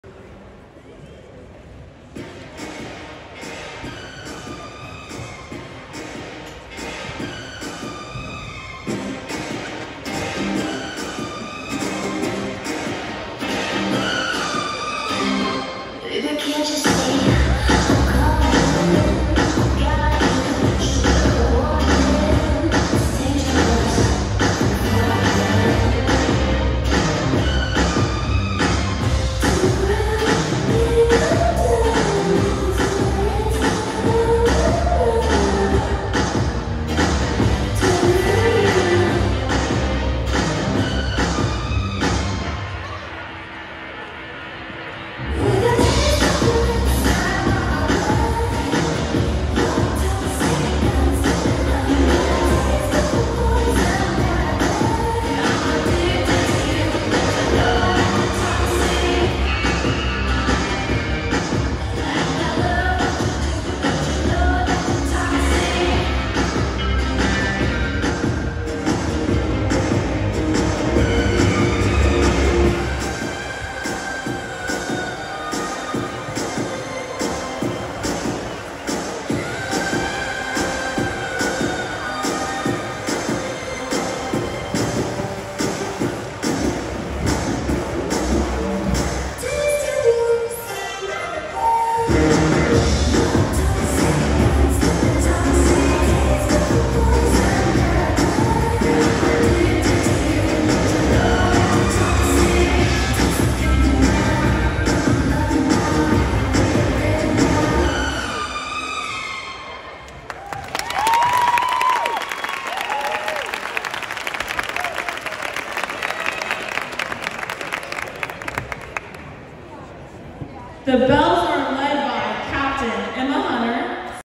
SHS Southern Belles 1-28-23
Southside High School Maverick Southern Belle Dance/Drill Team
Halftime Performance
Home game vs Rogers

The video is a performance of cheer/dance choreography which is enhanced by the accompanying or background music.